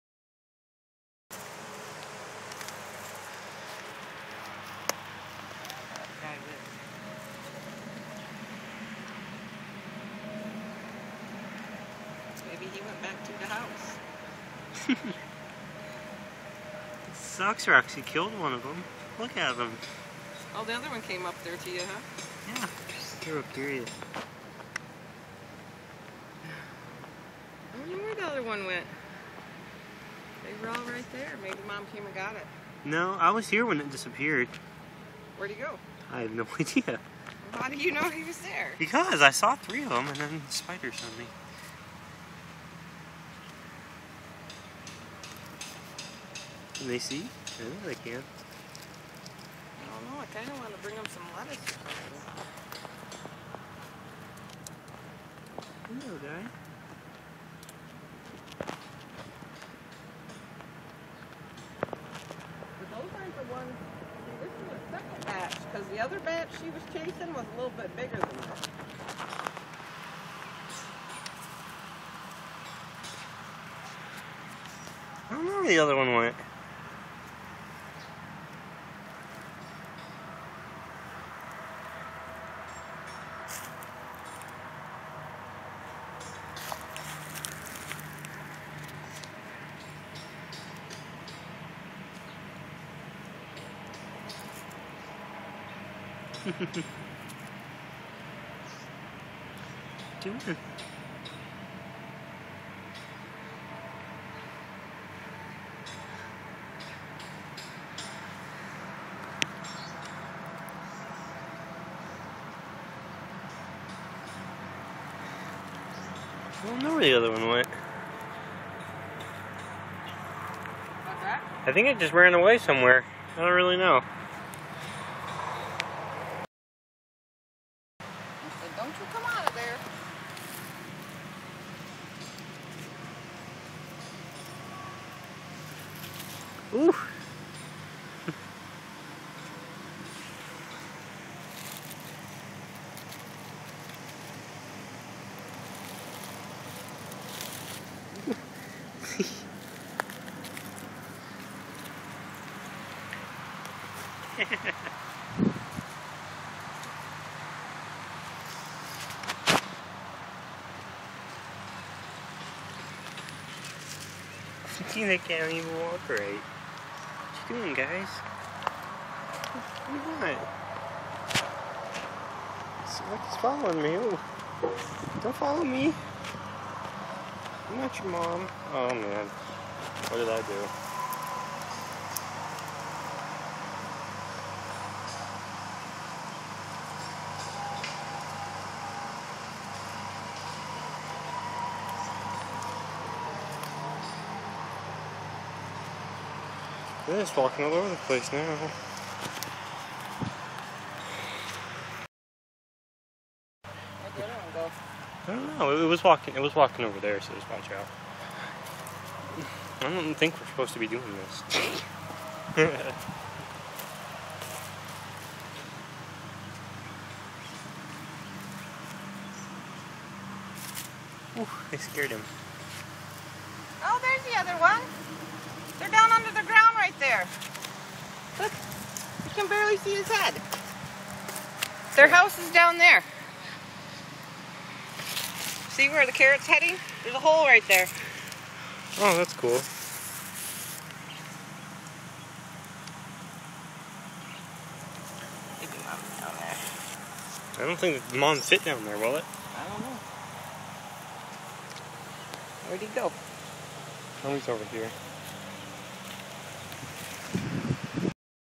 Baby Bunnies in our back yard!
After our dog killed one of the babies bunnys in our backyard we don't know if the mom is comming back or not. Here is some footage of it.